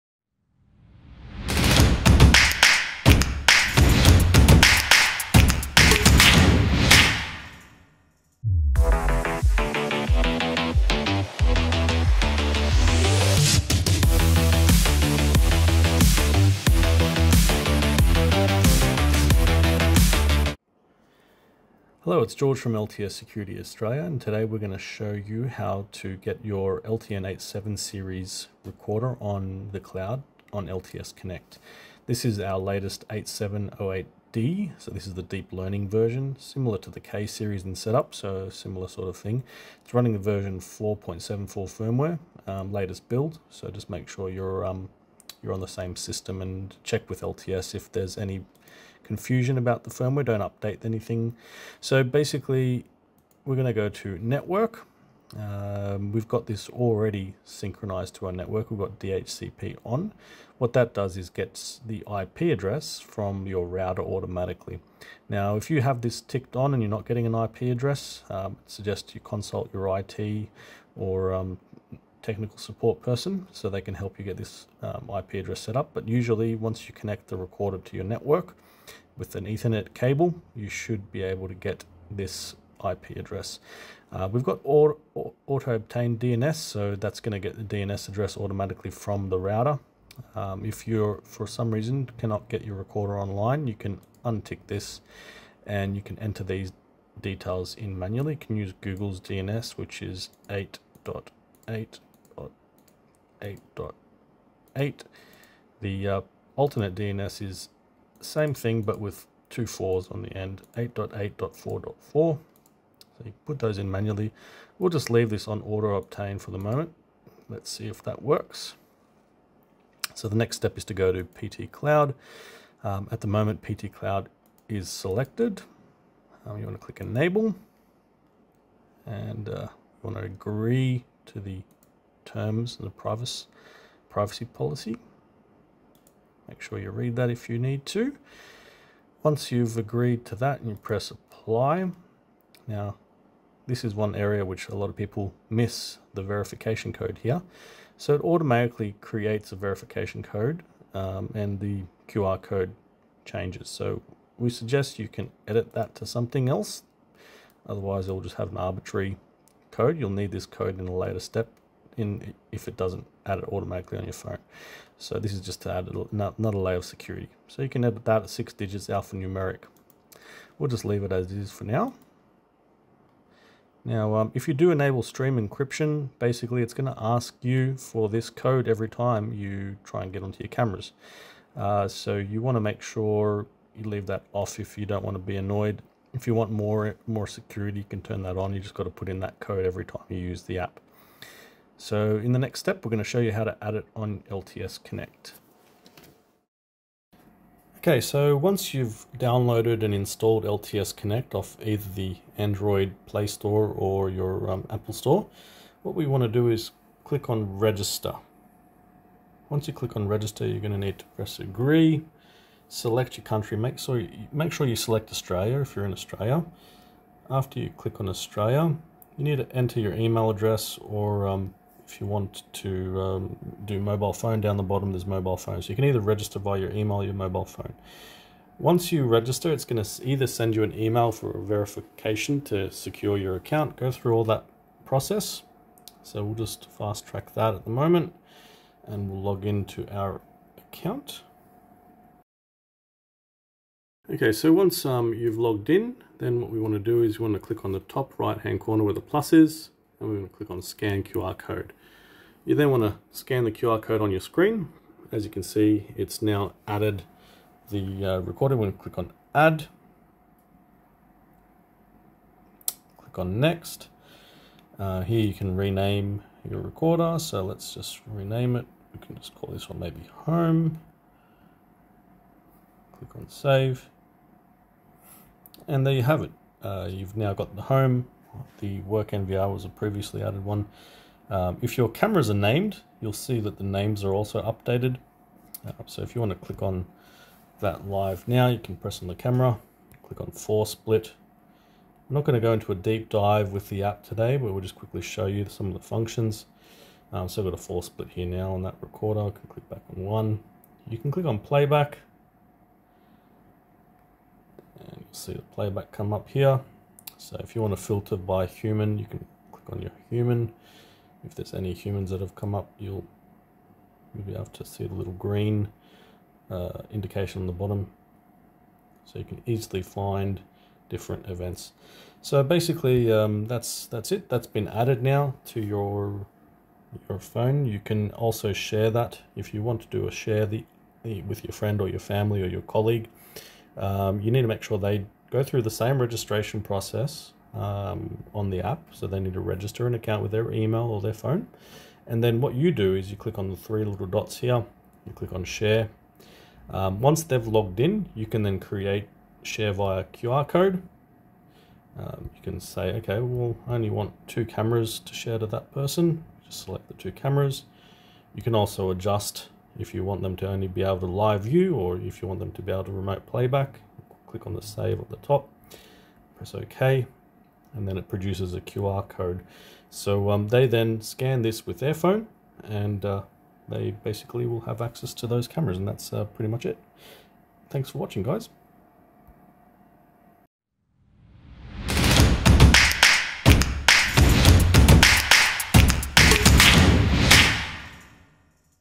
LTS Connect - 87 Series NVR - PT Cloud Guide
This video shows you how to connect your LTS 87 Series (D Series) NVR to LTS Connect, It also shows some basic functions on LTS Connect & how to easily share to Family or Colleagues. This is similar UI as the LTS K Series.

 / ltsaustraliaofficial

 / ltsaustrliaofficial